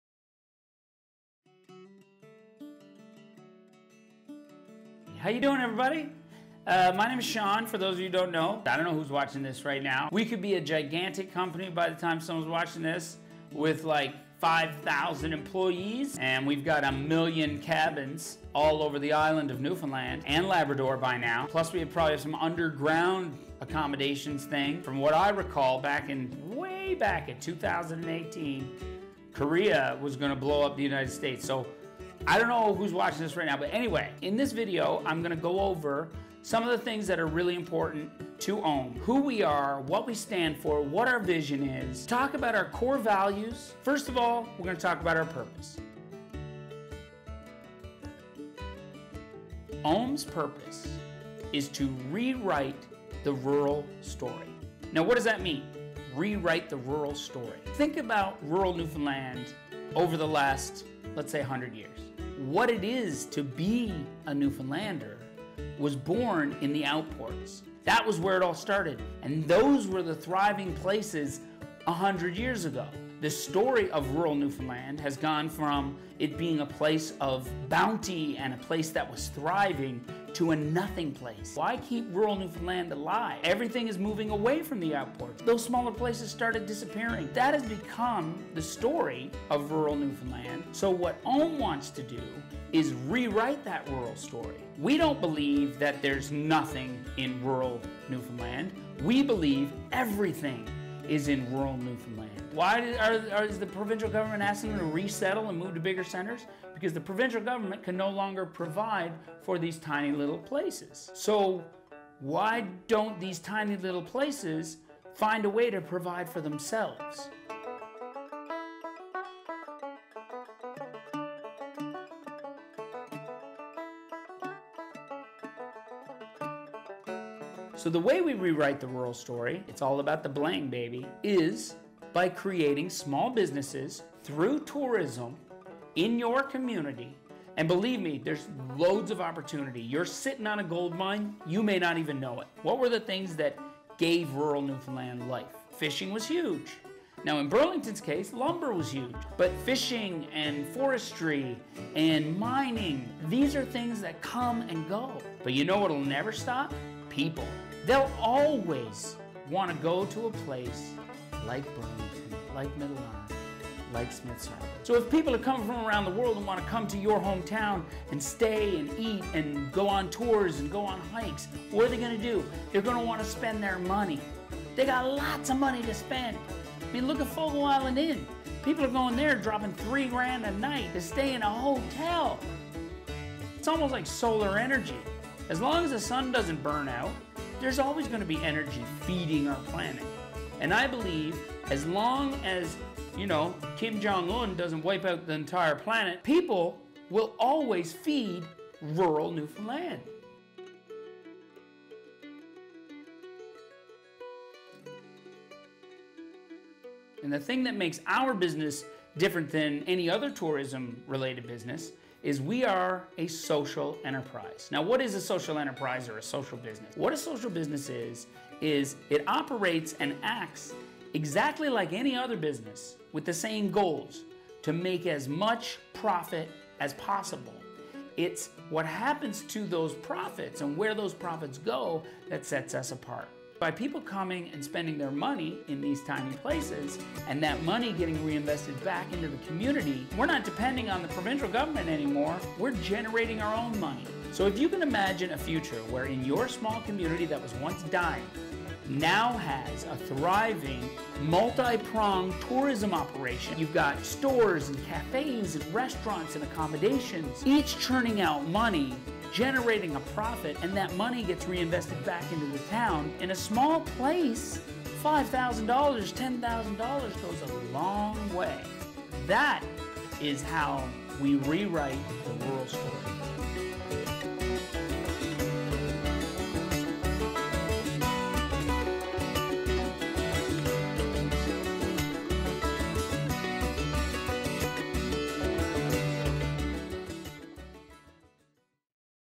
OME INTRO AND PURPOSE
An overview of the purpose of the Ome Social Enterprise in Burlington, Middle Arm and Smith's Harbour Newfoundland and Labrador.